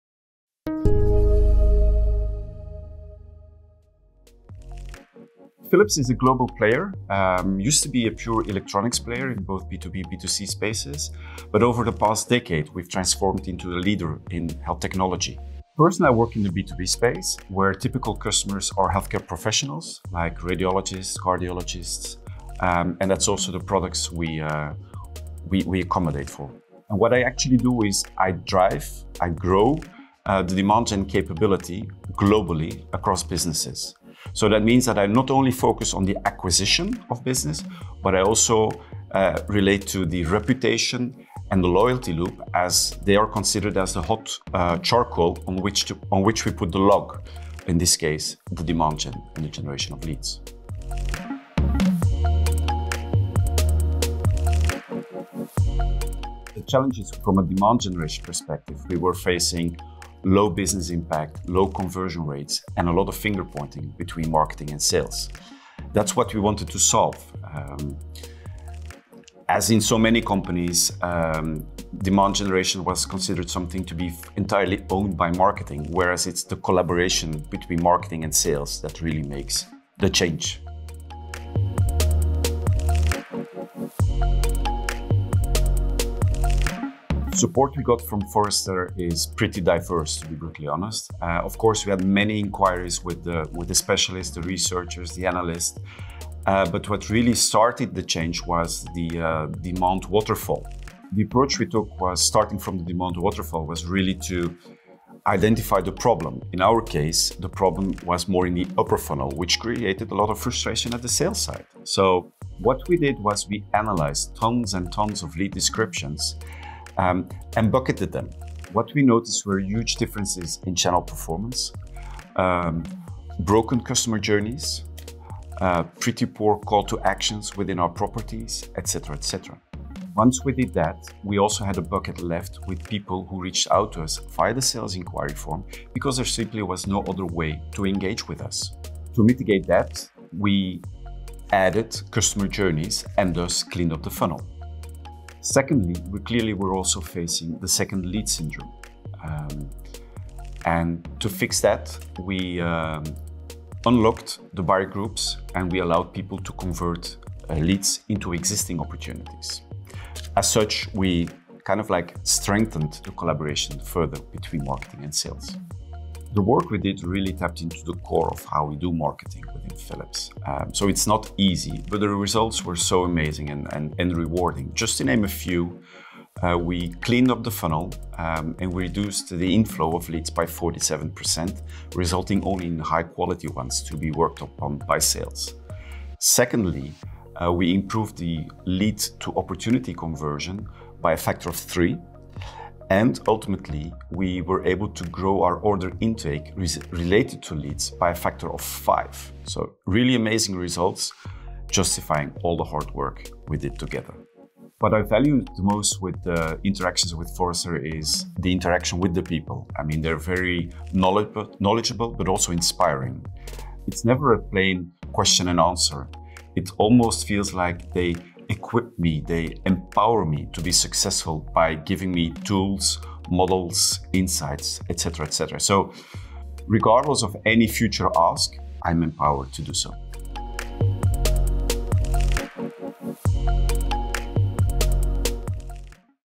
Rev The Demand Engine Through Tighter Marketing And Sales Alignment | A Client Success Story
Learn how working with Forrester helped Philips strengthen marketing and sales alignment and triple its lead-to-opportunity conversion rate.